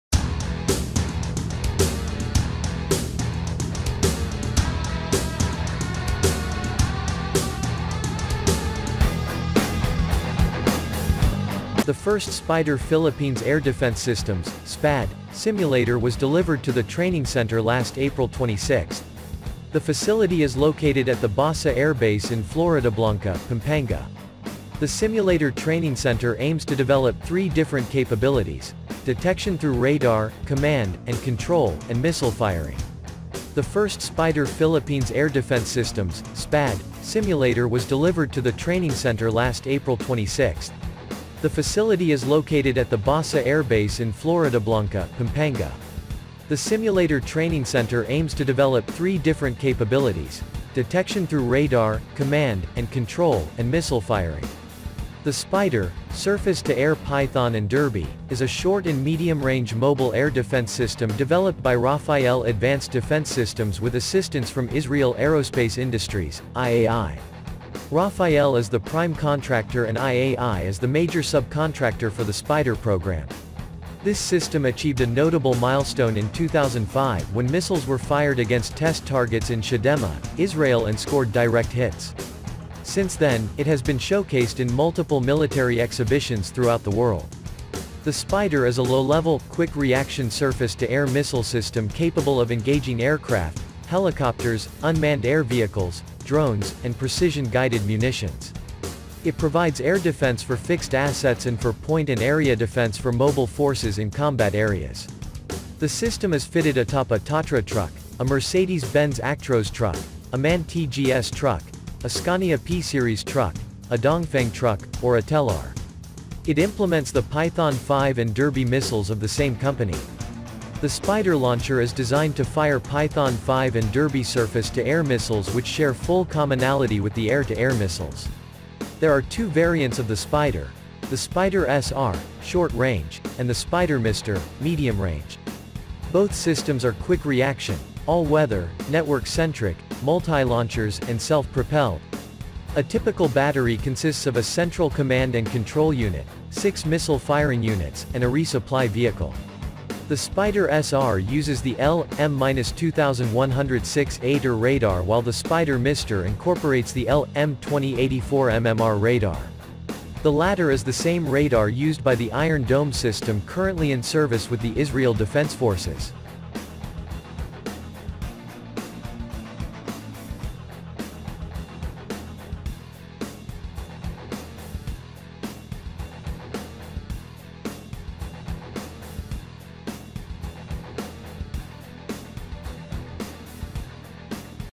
Philippines SPYDER Air Defense Missile System! Are you still going to buy the Long Range version?
The first Spyder Philippines Air Defense Systems (SPAD) simulator was delivered to the training center last April 26. The facility is located at the Basa Air Base in Floridablanca, Pampanga. The simulator training center aims to develop three different capabilities: detection through radar, command, and control, and missile firing.